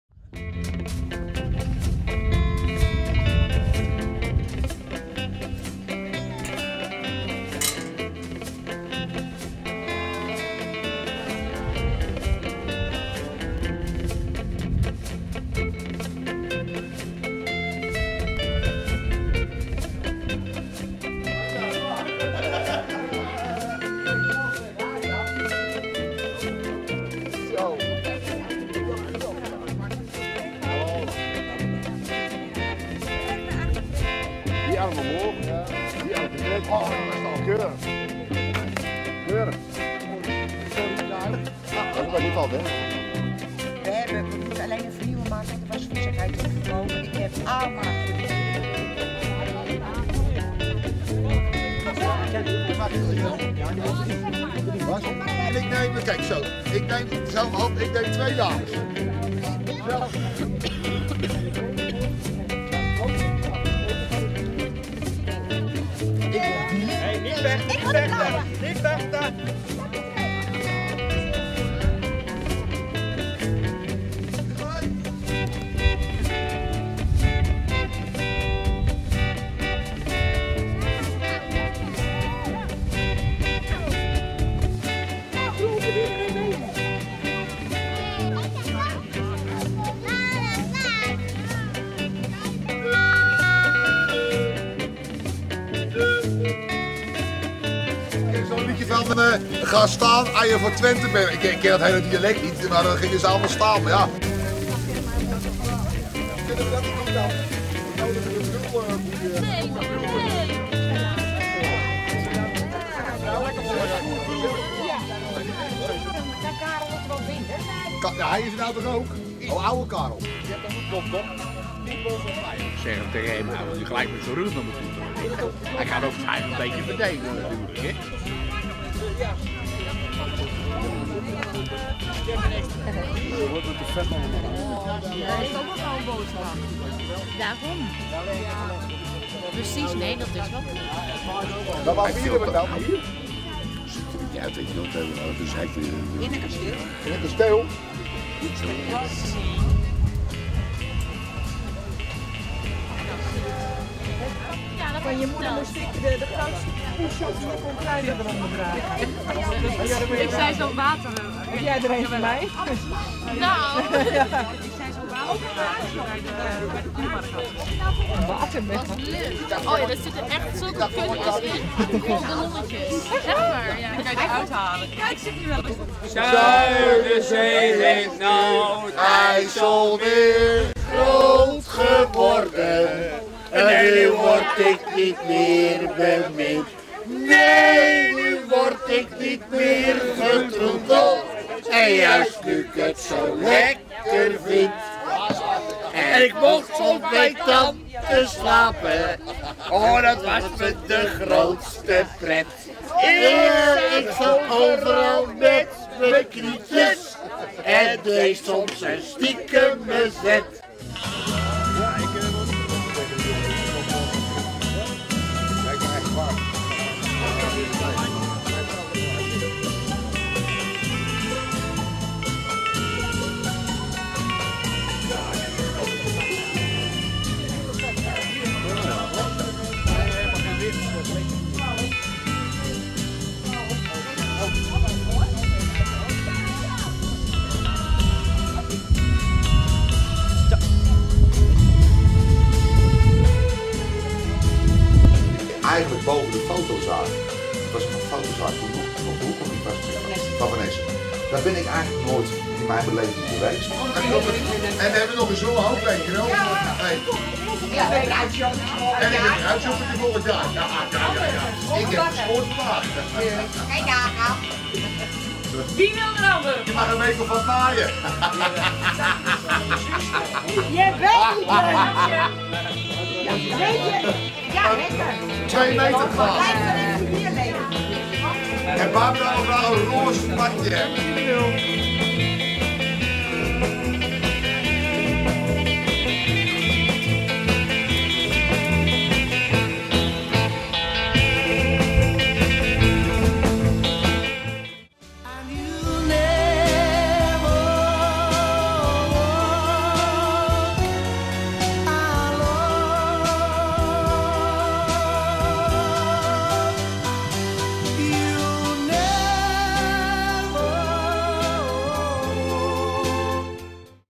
Familiedag van der Meij 2011.avi
Ook dit jaar weer een mooie familiedag  in Warnsveld bij Ineke en Leen